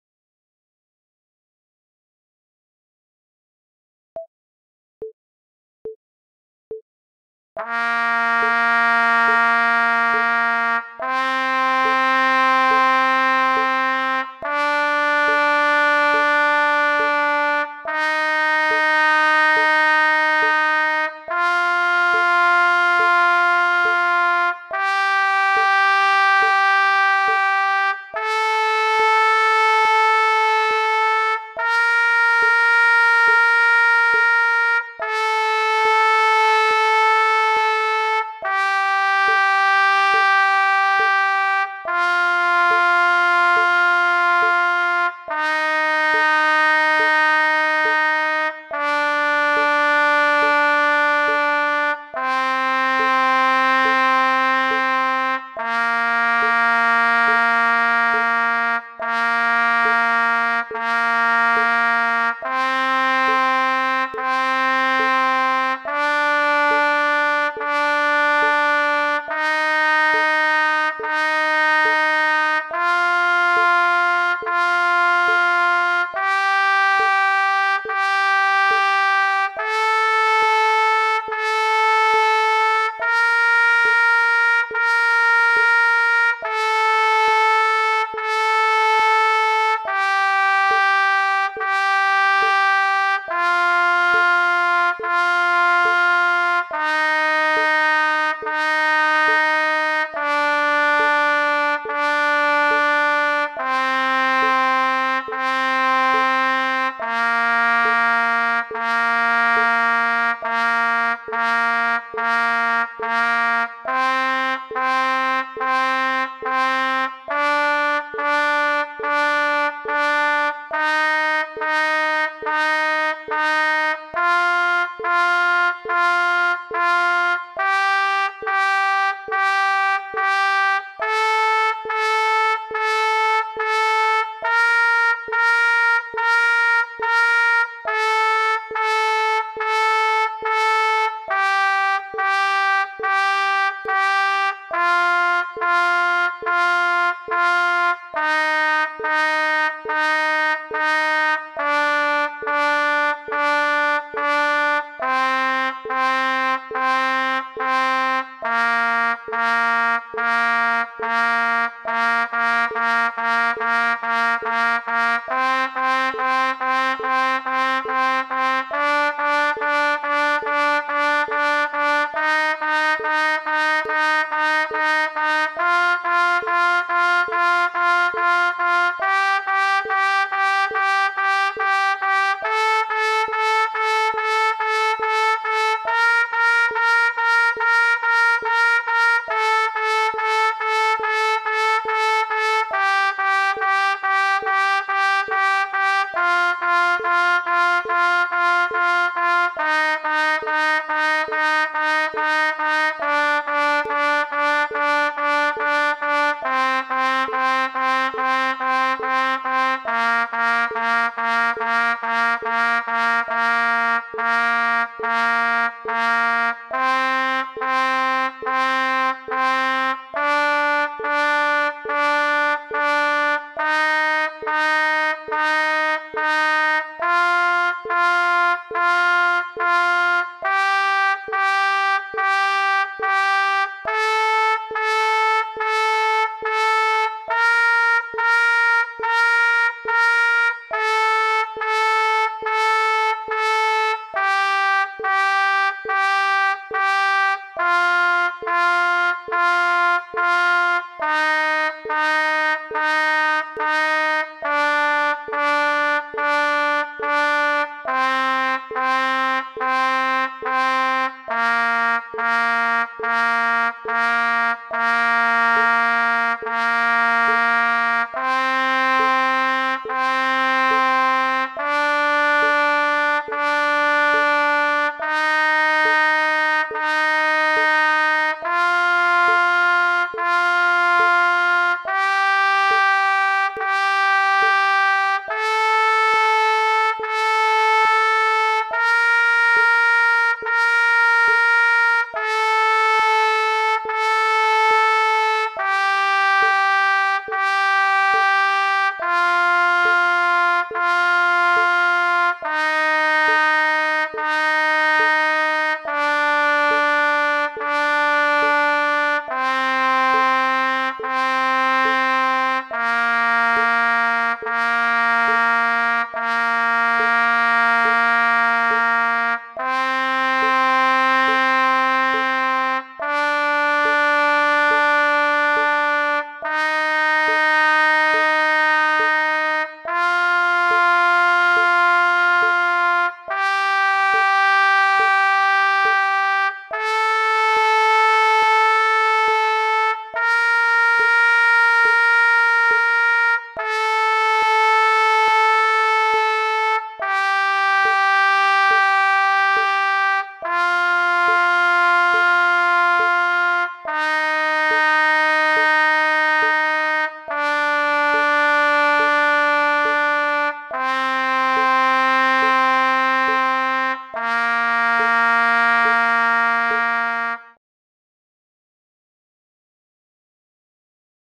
How to Play C Major Scale on Trumpet. Finger Positions and Rhythms
How to Play C Major Scale on Trumpet. Finger Positions and Rhythms. Learn to play quickly and easily.